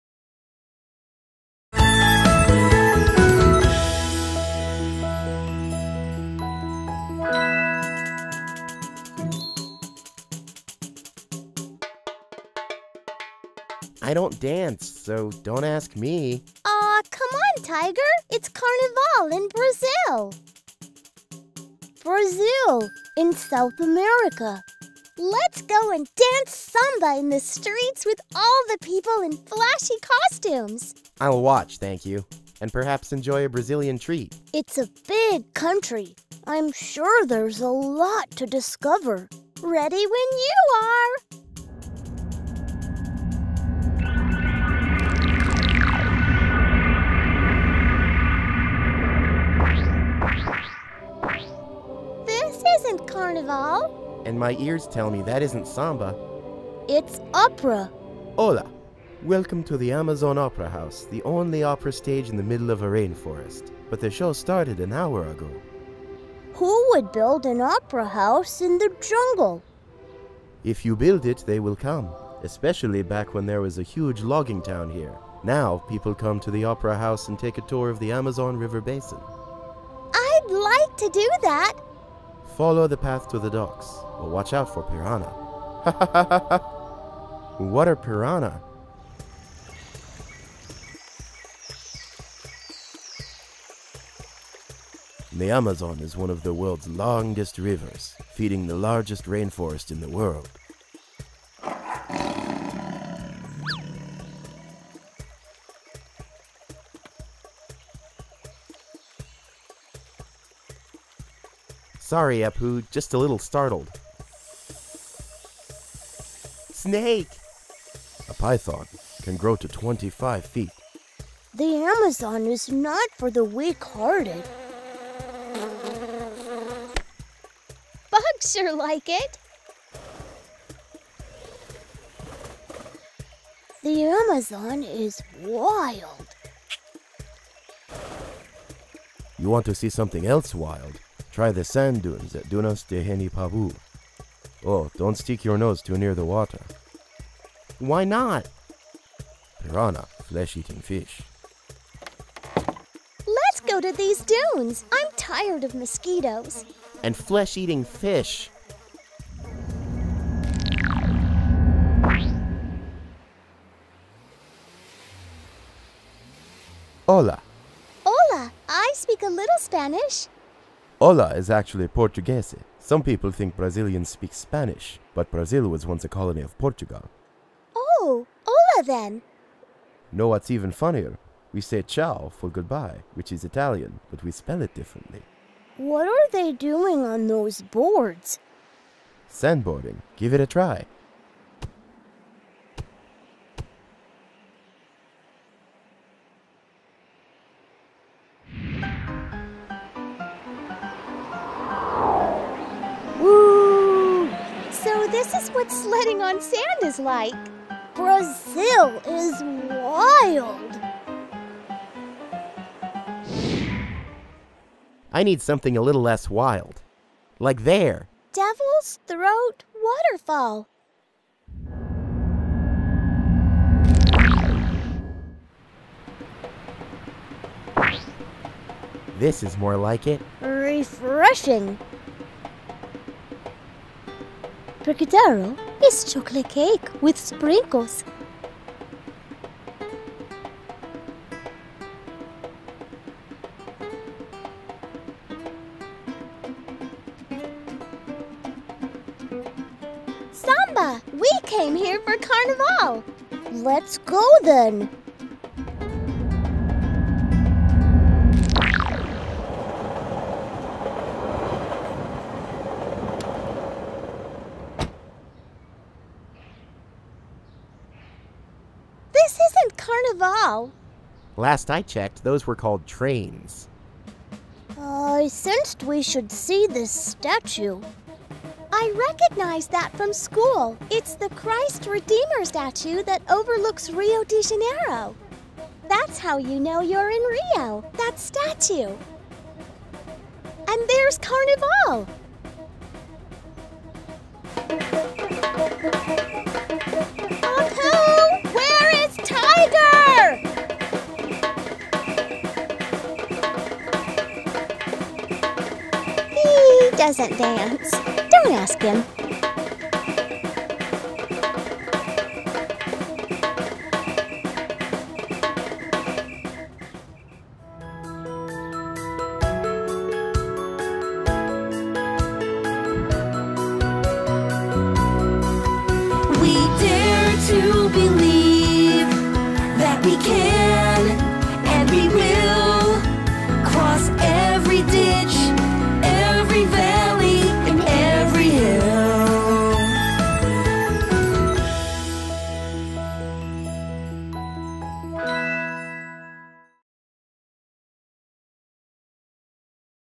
Short Stories for Kids - Trip to Brazil
The series 'Learn about Countries' has seen Appu, Neena and Tiger travel the world, to different places. Let us learn about the country where Samba originated. In this story about Brazil, Neena wishes to witness the carnival in the country.

The trio visits the Amazon Opera House, which is the only Opera stage in the middle of a rainforest. The guide explains that there is a lot of attraction from people despite being in the jungle, as people tour the Amazon River basin.

As they take a boat ride, they see the lush green side of Amazon, with a plethora of flora and fauna. They learn that the Amazon is not for the weak hearted after seeing a lot of wild animals.

The trio then has fun, sandboarding on the deserts of the country after learning that Brazil was once a colony of Portugal. The trio pays a visit to the Devil's Throat Waterfall, where they have some yummy food, before heading to the carnival.

They witness the Christ Redeemer statue, which overlooks the city of Rio de Janeiro. They go to the grand carnival, where they dance to their heart's content.